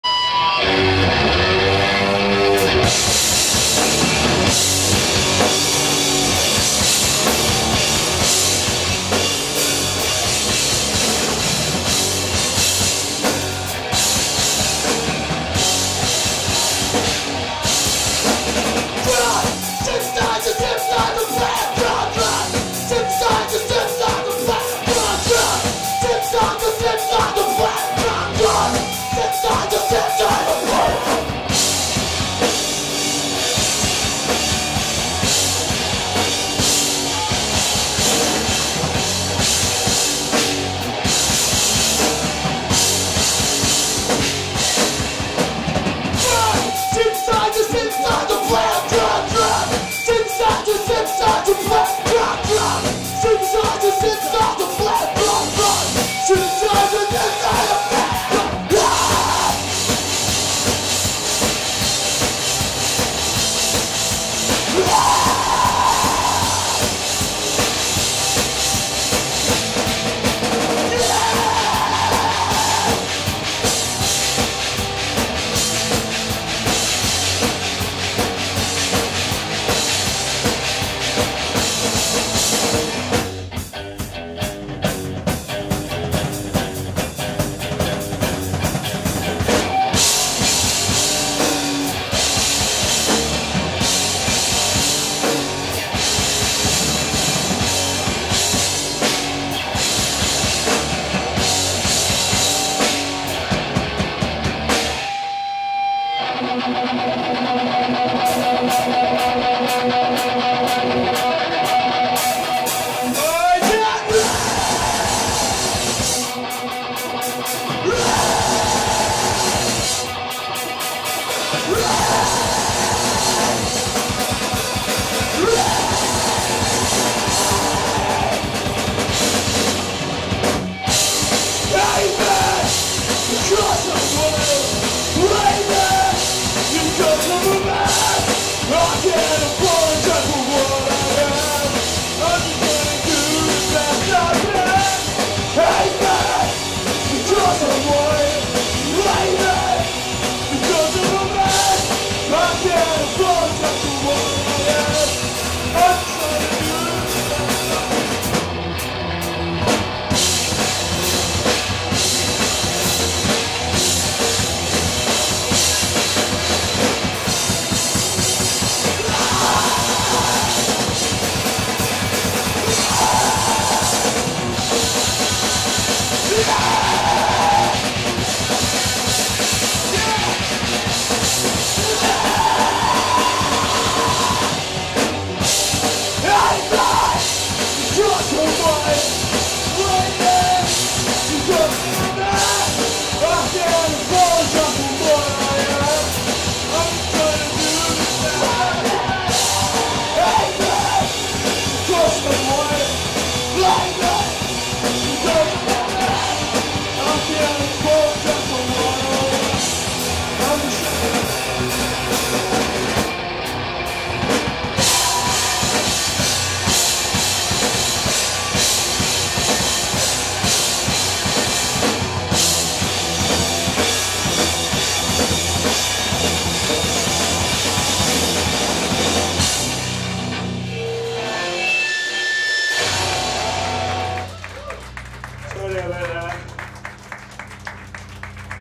Nae Joy 2 @ The 13th Note Glasgow Scotland 5/3/2019
Bloodstock Metal 2 The Masses 2019 Round 1 Heat 1 featuring Apathy's Edge, Nae Joy, Kyber, From Tyranny